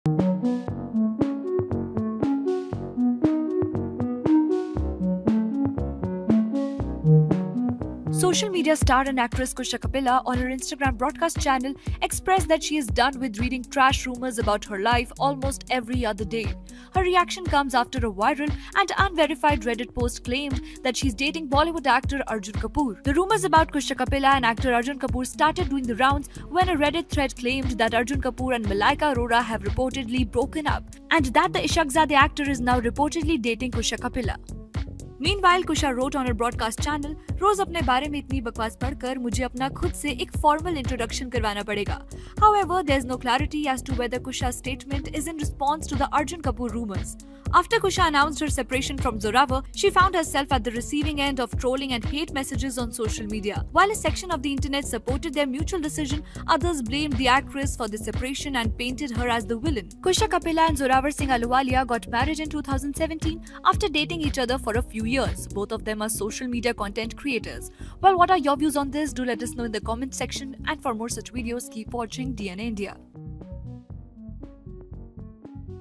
Kusha Kapila Is Dating Arjun Kapoor? Social Media Star Reacts To Her Dating Rumours With Arjun
Social media star and actress Kusha Kapila, on her Instagram broadcast channel, expressed that she is done with reading trash rumours about her life almost every other day. Her reaction comes after a viral and unverified Reddit post claimed that she is dating Bollywood actor Arjun Kapoor.